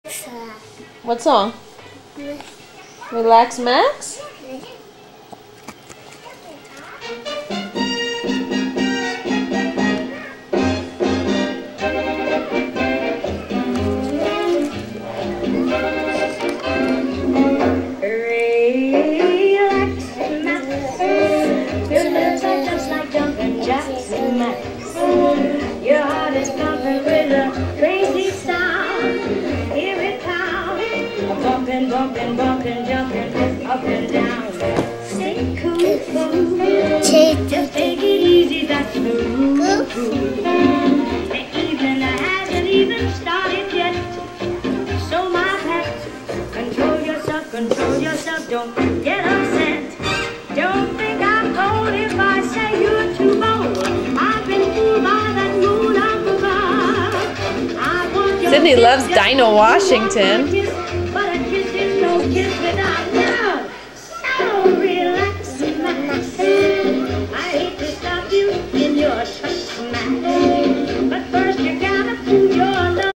Sydney Relax Max
Sydney singing her favorite song